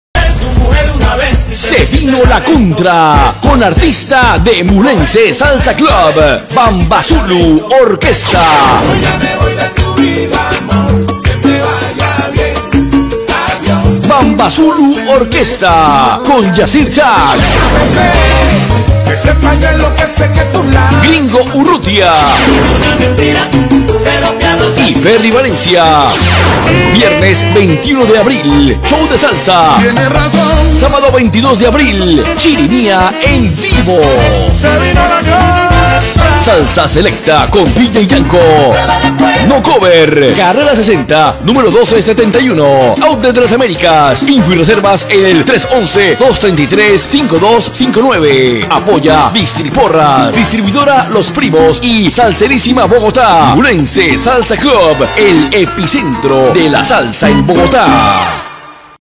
Evento Se Vino La Contra en Mulenze Salsa Club Bogotá
SE VINO LA CONTRA en Mulenze Salsa Club con Bambazulú Orquesta ~ El Gringo Urrutia ~ Yassir Sax & Ferry Valencia
Viernes 21 de Abril Show de Salsa
Sábado 22 de Abril Chirimia en Vivo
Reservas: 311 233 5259
Presenta: Salserisima Bogotá